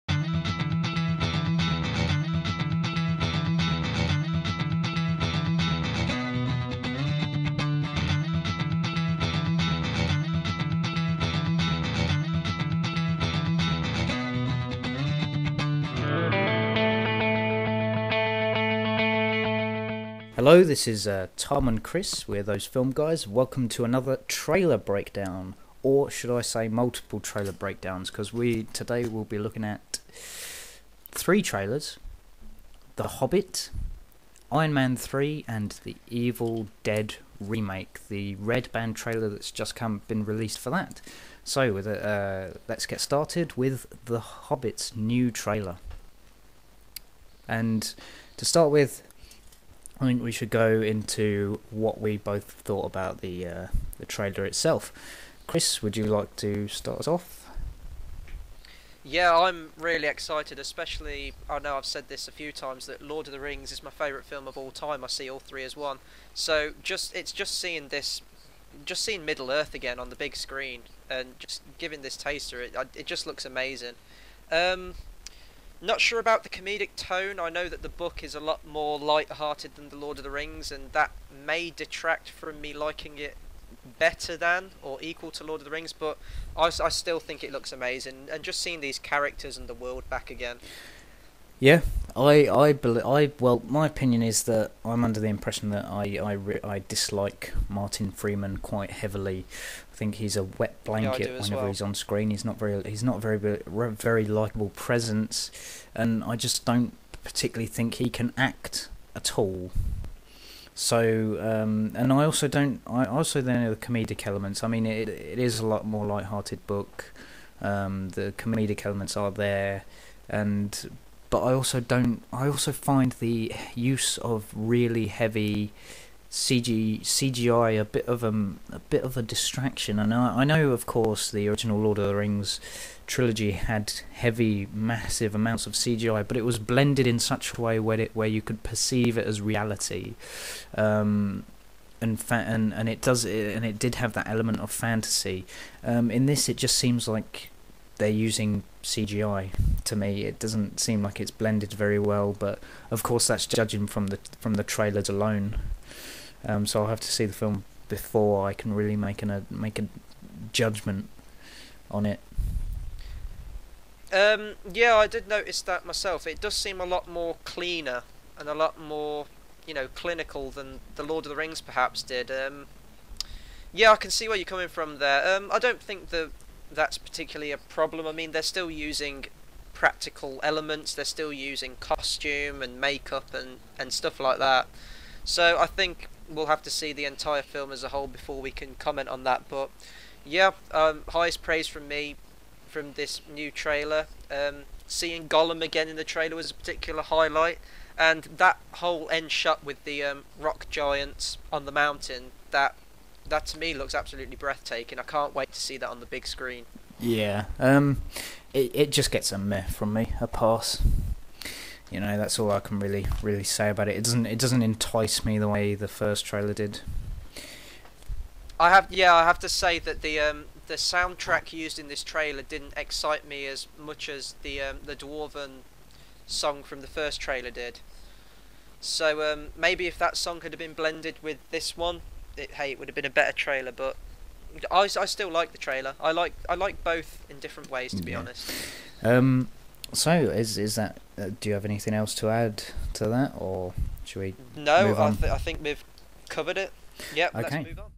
The Hobbit Trailer: : Source TV Review
The superb film review team of Chris and Tom bring you their fourth film, and third weekly, in their latest take on the latest films.

Don't forget to check out their youtube channel 'Those Film Guys'